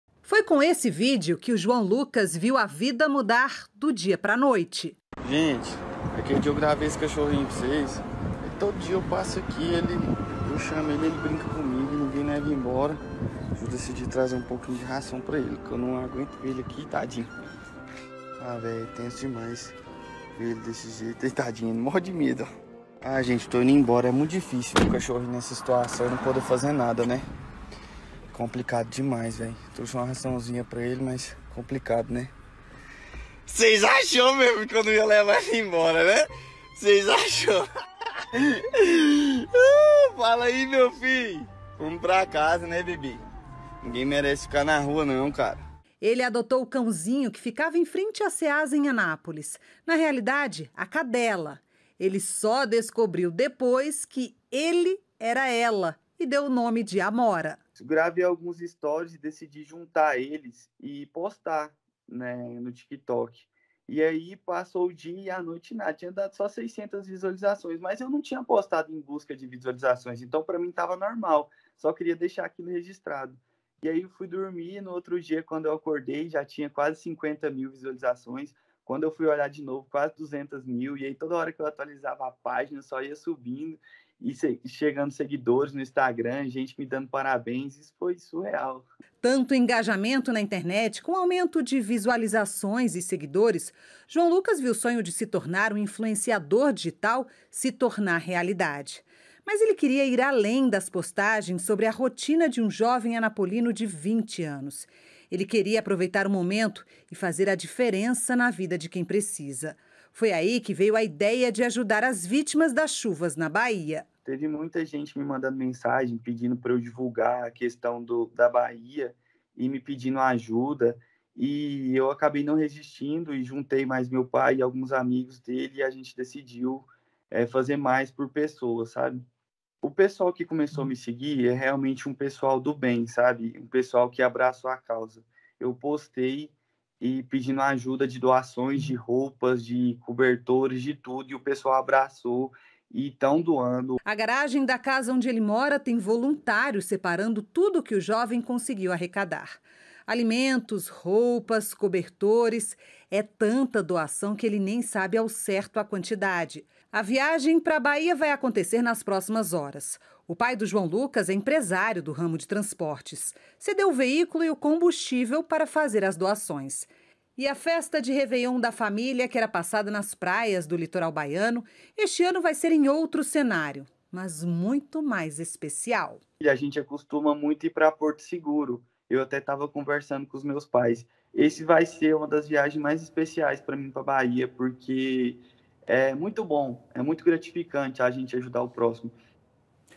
CORRENTE DO BEM: INFLUENCIADOR ARRECADA DOAÇÕES PARA A BAHIA
UM JOVEM ANAPOLINO, DE 20 ANOS, APROVEITOU A VISIBILIDADE QUE GANHOU EM UMA REDE SOCIAL, DEPOIS DE POSTAR UM VÍDEO EM QUE ADOTAVA UMA CADELA PARA FAZER MAIS. COM O NÚMERO DE VISUALIZAÇÕES E SEGUIDORES CRESCENDO, ELE RESOLVEU AJUDAR AS VÍTIMAS DAS CHUVAS NA BAHIA.